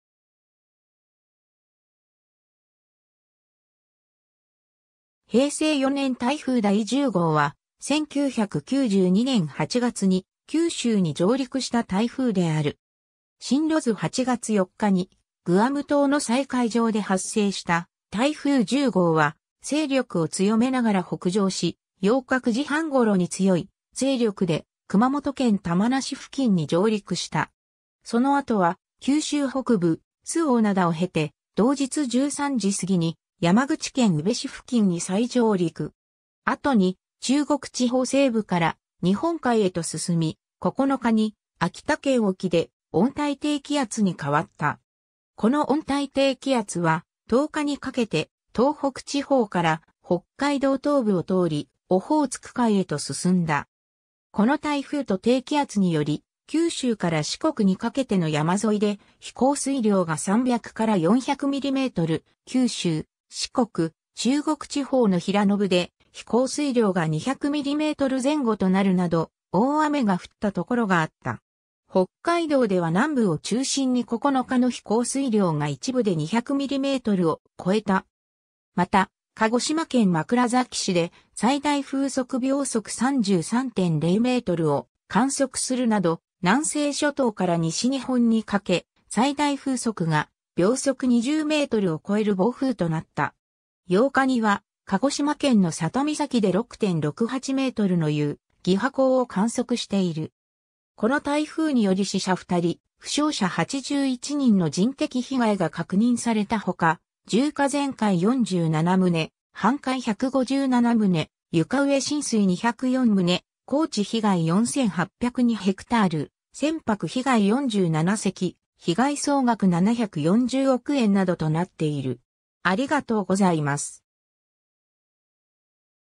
平成4年台風第10号
平成4年台風第10号（へいせい4ねんたいふうだい10ごう、国際名：ジャニス/Janis）は、1992年8月に九州に上陸した台風である。
 進路図 8月4日にグアム島の西海上で発生した台風10号は、勢力を強めながら北上し、8日9時半頃に強い勢力で熊本県玉名市付近に上陸した。
 その後は九州北部、周防灘を経て、同日13時過ぎに山口県宇部市付近に再上陸。
後に中国地方西部から日本海へと進み、9日に秋田県沖で温帯低気圧に変わった。
 この温帯低気圧は、10日にかけて東北地方から北海道東部を通り、オホーツク海へと進んだ。
 この台風と低気圧により、九州から四国にかけての山沿いで日降水量が300～400mm、九州・四国・中国地方の平野部で日降水量が200mm前後となるなど、大雨が降った所があった。
 北海道では南部を中心に９日の日降水量が一部で200mmを超えた。
 また、鹿児島県枕崎市で最大風速33.0m/s（最大瞬間風速57.0m/s）を観測するなど、南西諸島から西日本にかけ最大風速が20m/sを超える暴風となった。
 ８日には、鹿児島県の佐多岬で6.68ｍの有義波高を観測している。
 この台風により死者2人・負傷者81人の人的被害が確認されたほか、住家全壊47棟、半壊157棟、床上浸水204棟、耕地被害4802 ha、船舶被害47隻、被害総額740億円などとなっている。